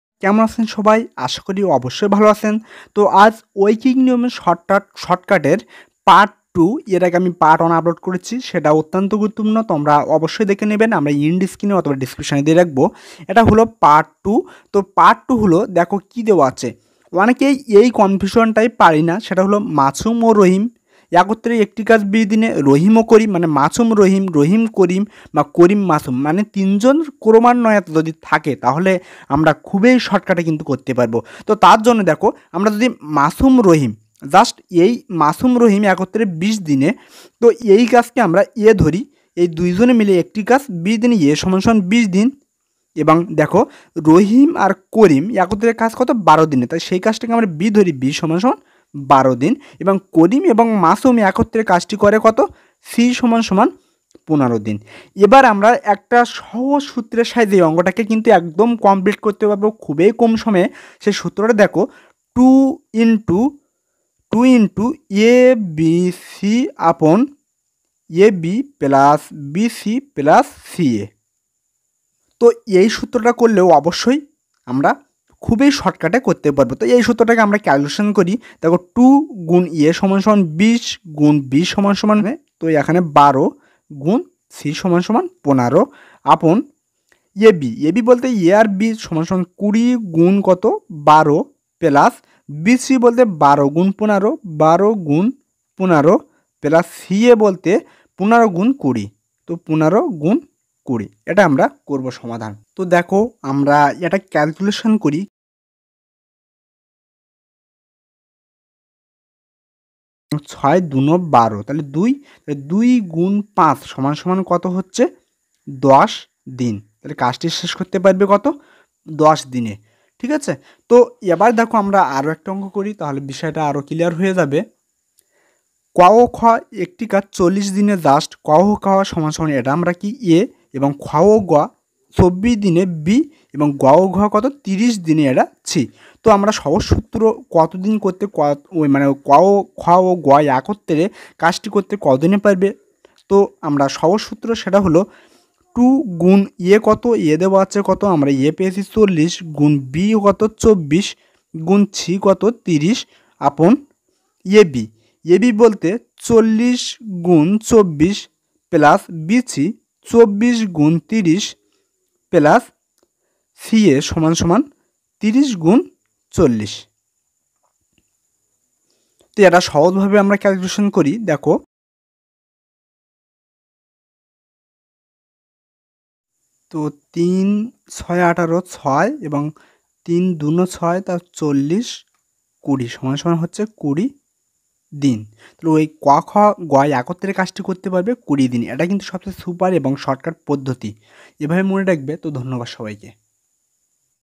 ঐকিক নিয়মের অংক || oikik niyom math shortcut || short method solution
ঐকিক নিয়মের অংক || oikik niyom math shortcut || short method solution || part :2

আজ ঐকিক নিয়মের শর্টকাট অংক নিয়ে আলোচনা করব এটা পার্ট :২। এই ঐকিক নিয়মের অংক বিসিএসসহ যে কোন চাকরি পরীক্ষার জন্য অত্যন্ত গুরুত্বপূর্ণ। আজ আমরা ঐকিক নিয়ম খুবই মজা করে শিখব এবং খুবই সর্টকাটে। আশা করি এই ভিডিওটা দেখার পরে আর কোন ঐকিক নিয়মের অংক করতে সমস্যা হবে না।

ঐকিক নিয়মের অংক || oikik niyom math shortcut : part 1